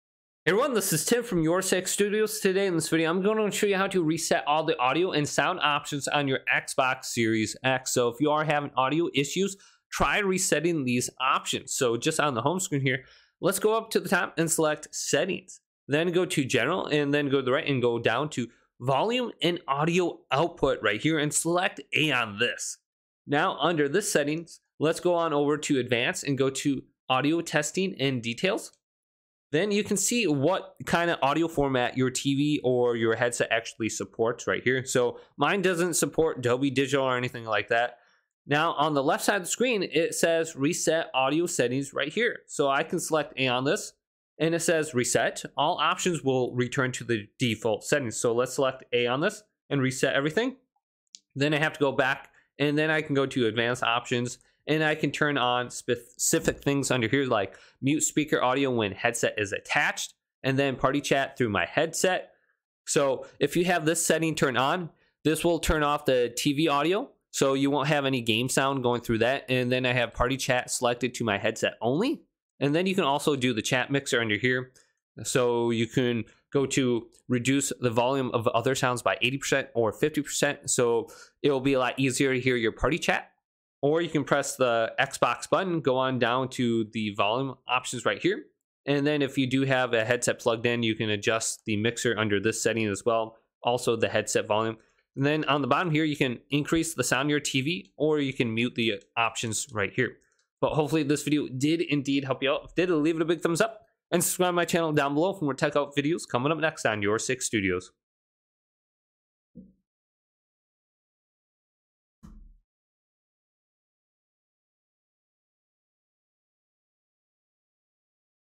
How to Reset Audio Settings & Fix Sound Issues on Xbox Series X (Fast Tutorial)
To reset audio settings and fix sound issues on Xbox Series X, you can follow the steps below:

Select Profile & system - Settings - General - Volume & audio output.
Select the Audio setup option under Advanced.
Highlight the audio setup option you wish to use and then press the A button to begin the audio test.
If the above steps do not resolve the issue, you can try the following solutions:

Check your TV and HDMI connections.
Run the Audio setup tool.
Troubleshoot when game or app audio is inaudible during voice chat.
Troubleshoot sound issues with an audio-video receiver.
Troubleshoot audio delay issues with an audio-video receiver or soundbar.